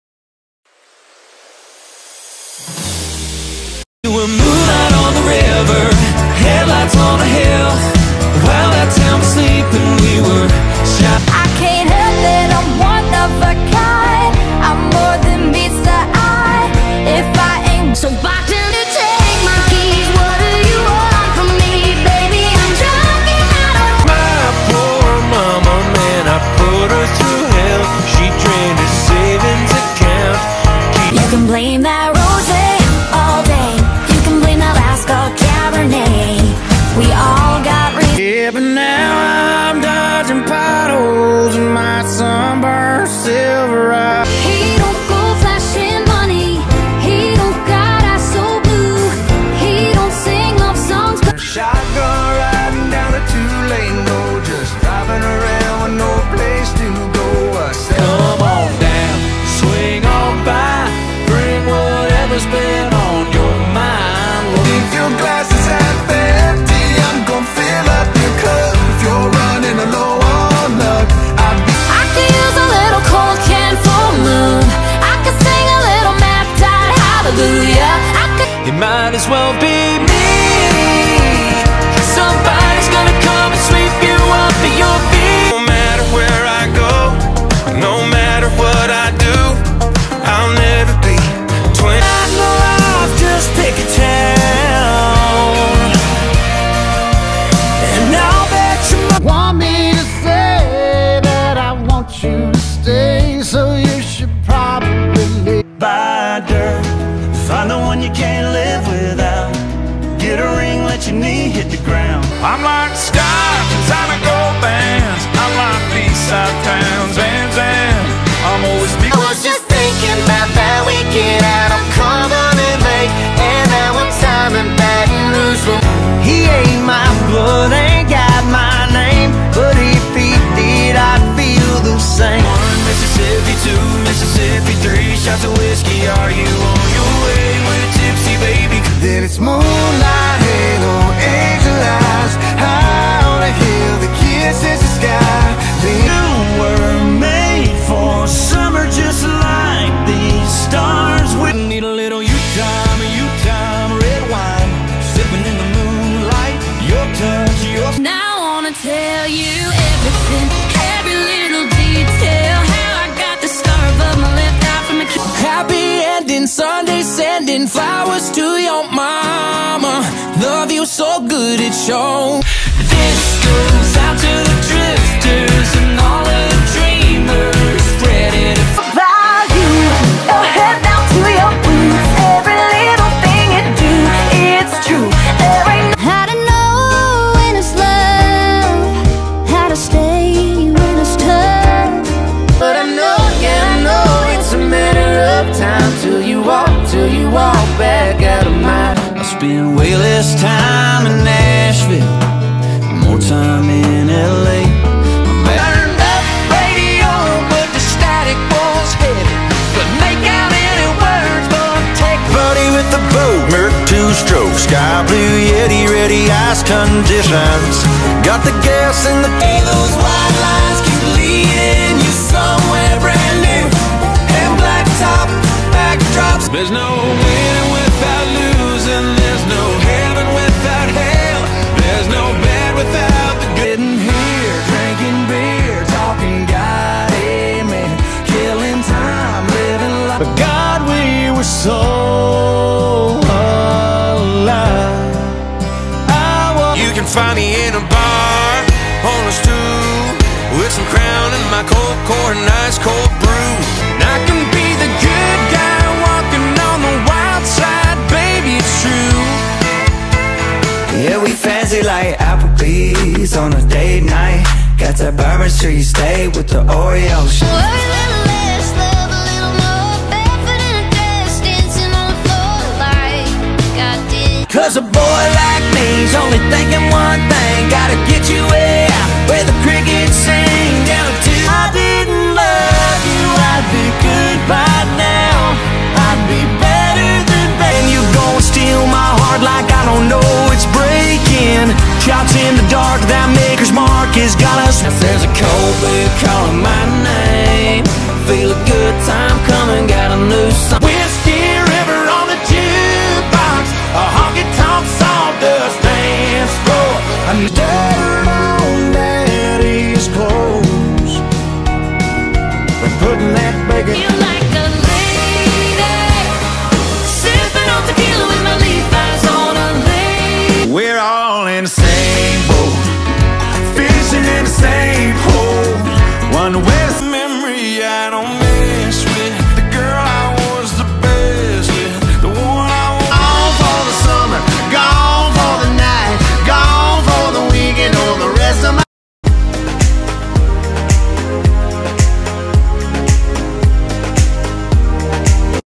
Billboard Top 50 Canada Country Chart (Oct 9 2021)
Billboard Canada Country Chart: Top 50 country songs in Canada on a week.